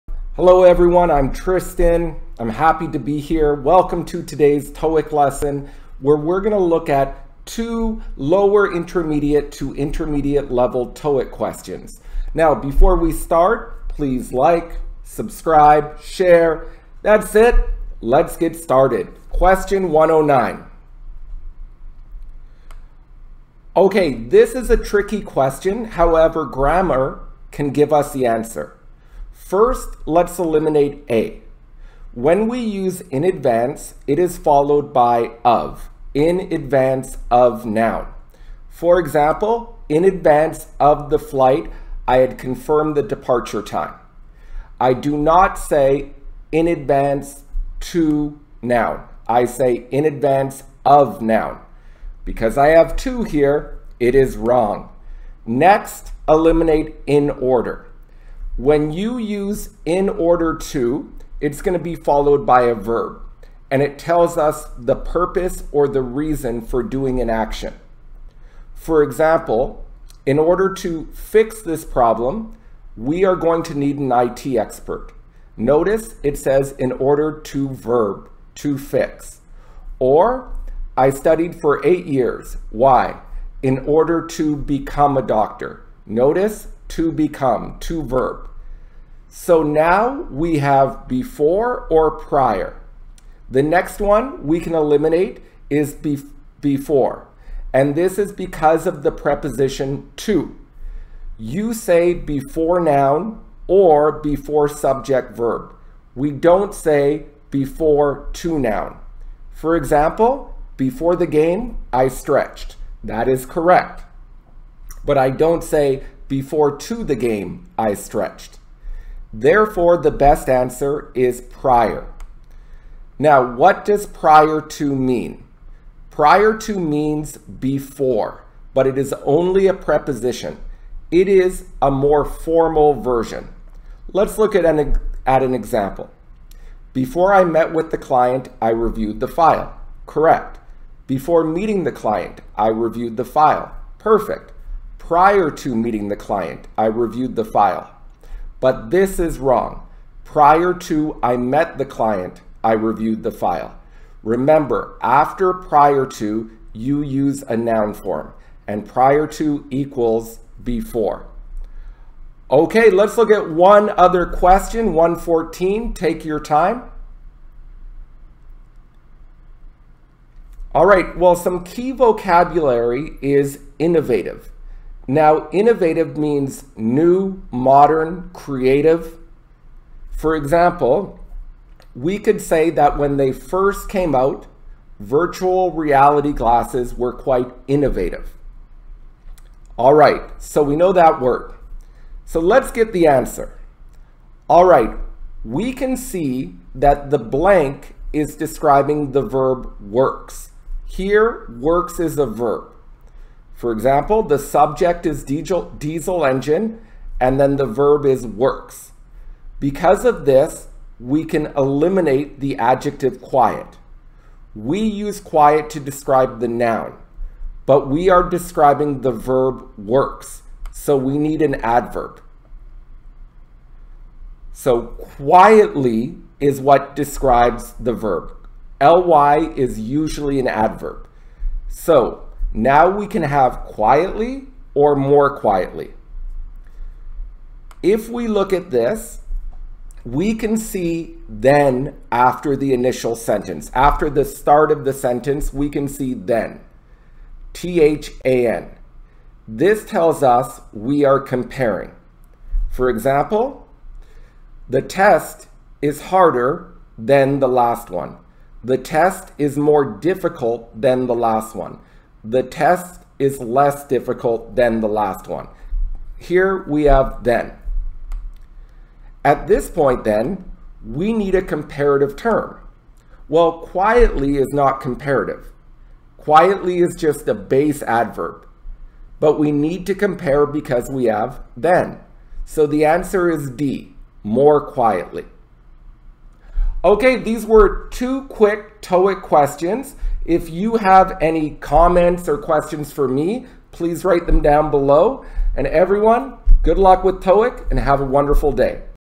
TOEIC 990 OR CLOSE: ANSWERING 2 TOEIC QUESTIONS#88
It's time to improve your TOEIC score.  This shows you easy ways to answer 2 questions.